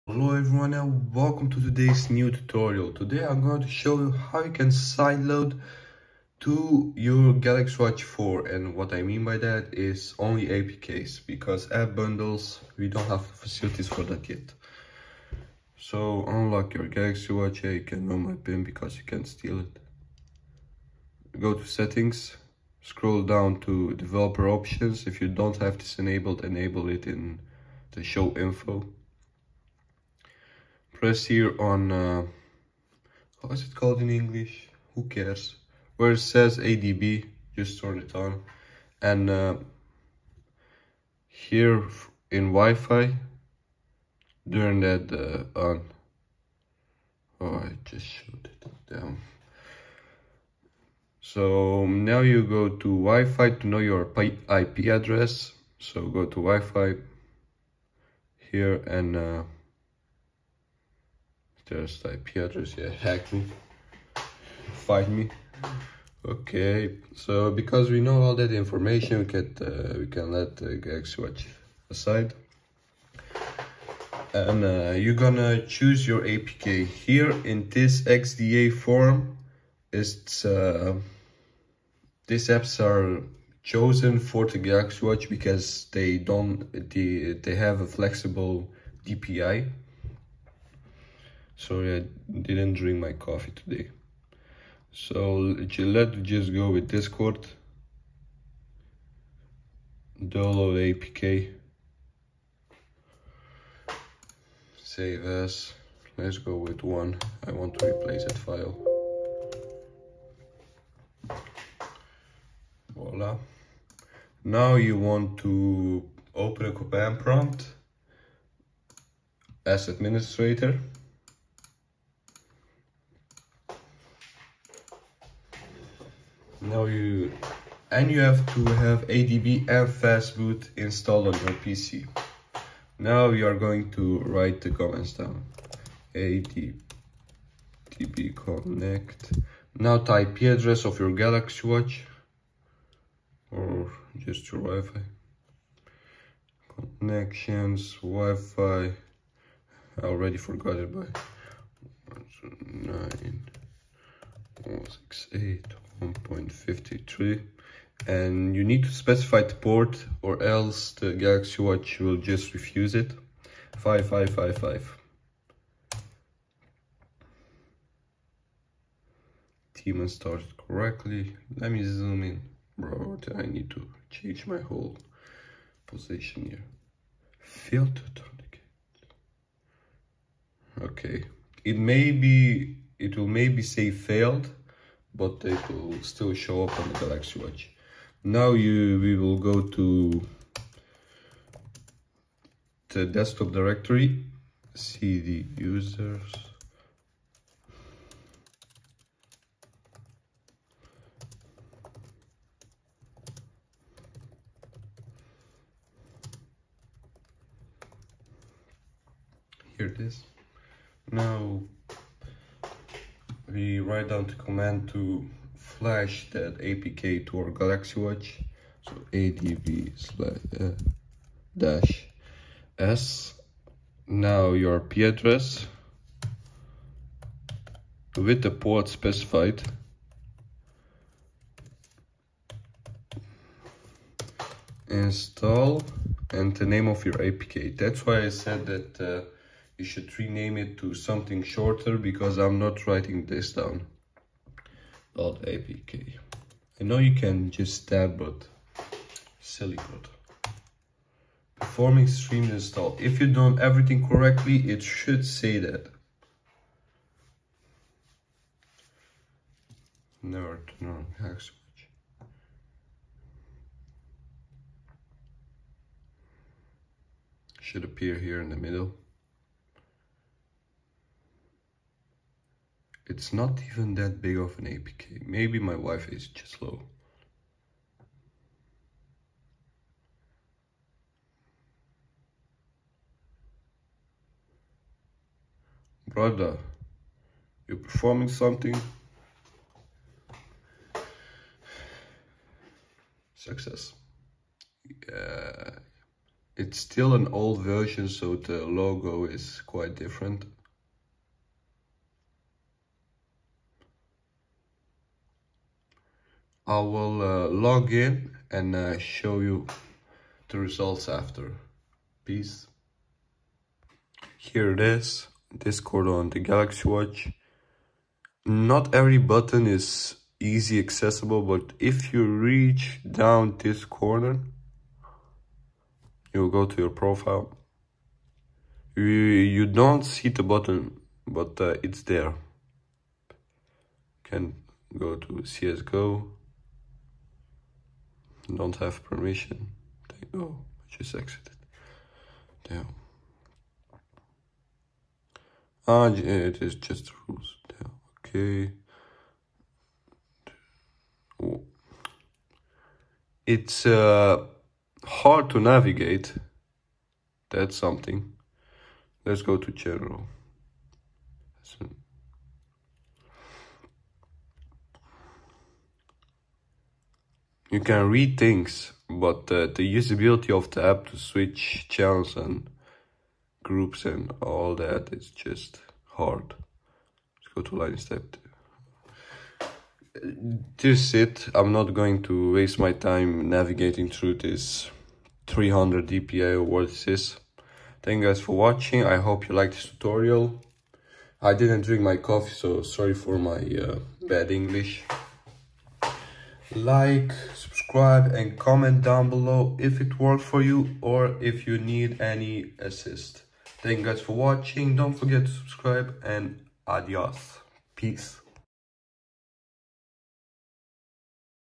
Sideloading Tutorial for Galaxy Watch4
How to sideload apps to your Wear OS watch | How to download the APKs

In this tutorial i will show you how to sideload apps with a PC.

▬ STAY UP-TO-DATE! ▬